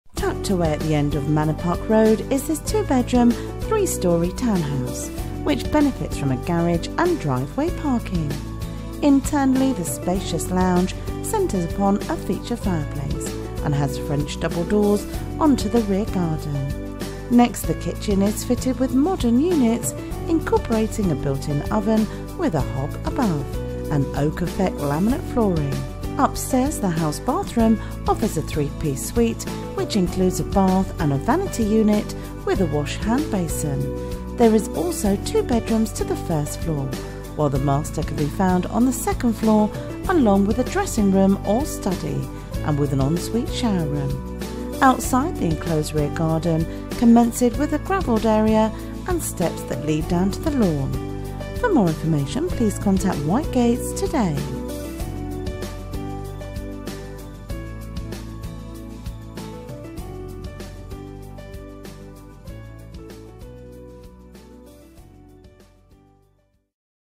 Bridon Way, Cleckheaton, West Yorkshire
** NO UPPER CHAIN ** Only by an internal inspection could you appreciate the true attributes of this superb light and airy three storey modern TOWNHOUSE, with THREE SPACIOUS DOUBLE BEDROOMS, TWO BATHROOMS and a GARAGE. Enviably located tucked away at the end of Manor Park Road it is handily placed for the everyday amenities offered by the town centre, and has excellent access to the motorway network links. The well presented and spacious accommodation is arranged over three floors and has gas fired central heating, 'Georgian' style UPVC double glazing and alarm. It briefly comprises of; ground floor entrance hall, cloaks/w.c., modern fitted kitchen, and spacious 16'4" x 13'3" lounge. To the first floor there are two double bedrooms and a modern family bathroom with three piece white suite, and the second floor boasts the impressive master suite with spacious 14'4" x 13'4" double bedroom, 10'4" x 6'3" dressing room/study and en-suite shower room with three piece white suite. This lovely